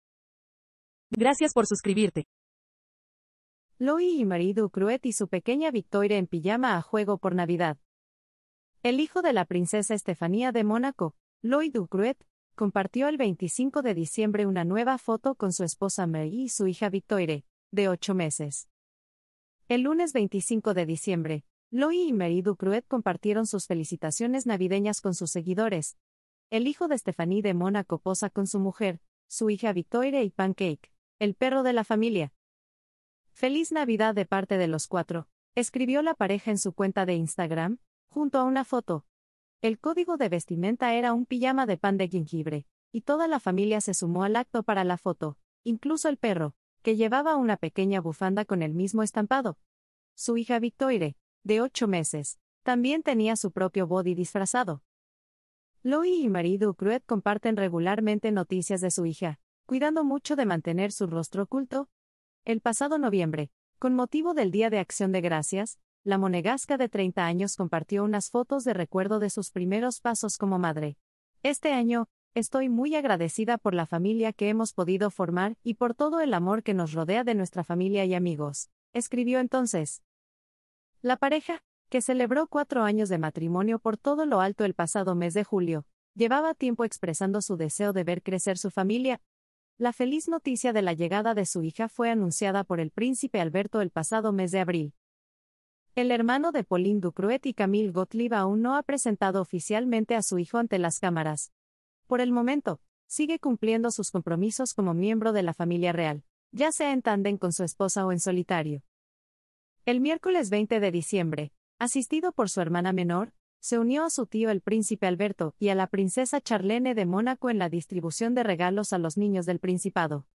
Louis y Marie Ducruet y su pequeña Victoire en pijama a juego por Navidad...
El hijo de la princesa Estefanía de Mónaco, Louis Ducruet, compartió el 25 de diciembre una nueva foto con su esposa Marie y su hija Victoire, de ocho meses.